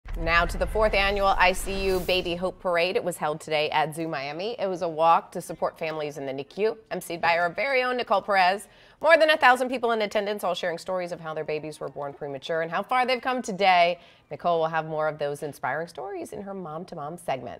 4th annual ICU Baby Hope parade held at Zoo Miami
ICU Baby helps support families with babies in the NICU